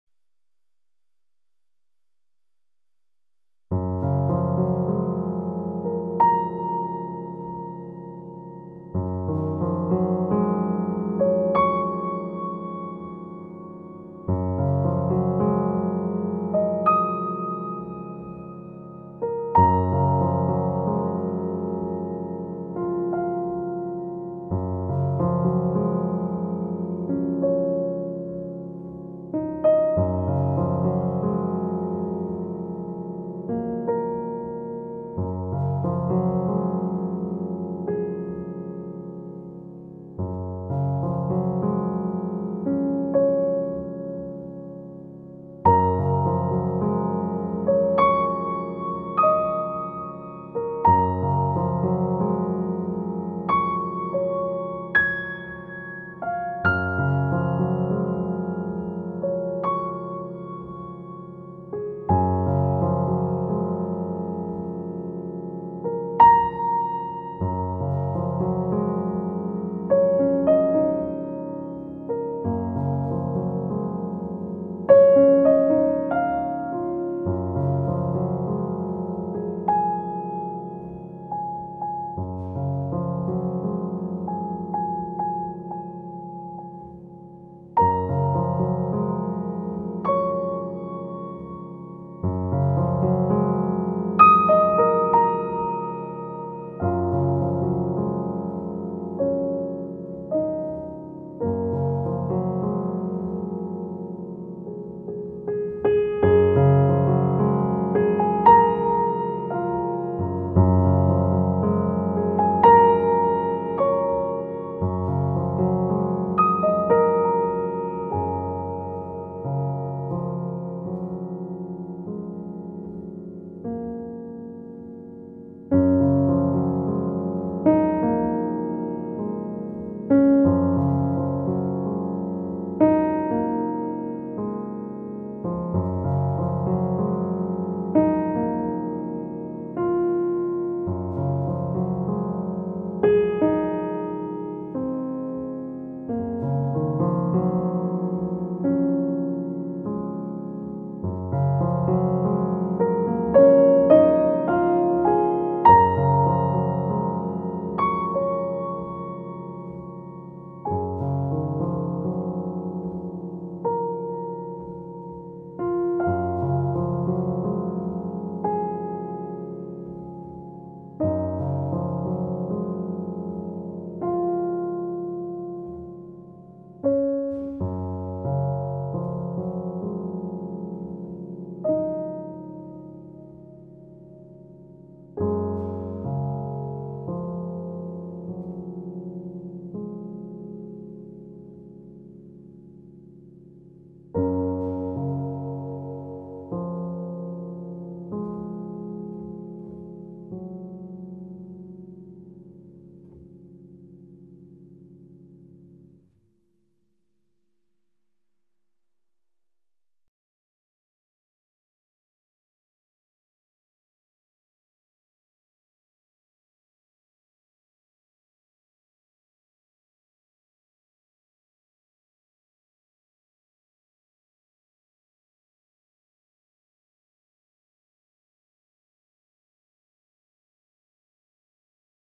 20210316 Piano Improvisation and Time-Lapse Video by Dan Newport
I recorded the music on January 17, 2021, on a Kawai MP11SE keyboard. The time-lapse video I made shooting out the window of my house on December 10, 2020, using a Google Pixel XL phone.